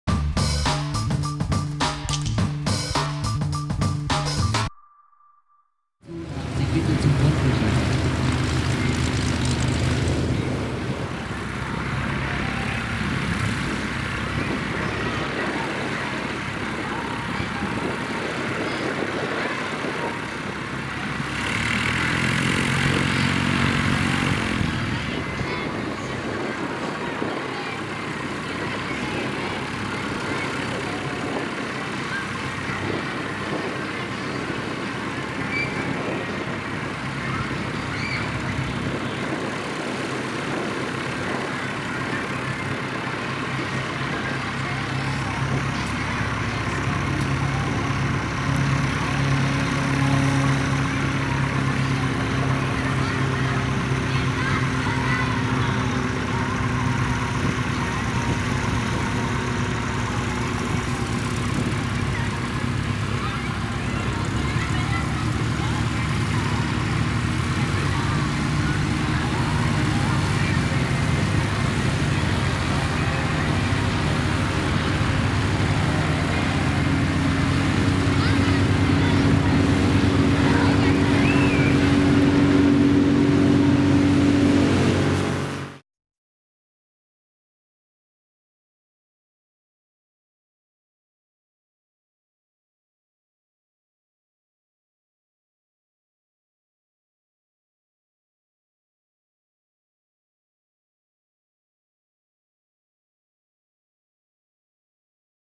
DA40 Diamond Star landing at Airfield Ferlach | D-ENWQ
Flugshow 2018 at Airfield Ferlach.

Airfield Ferlach | LOKG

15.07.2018

Diamond DA-40 Diamond Star
D-ENWQ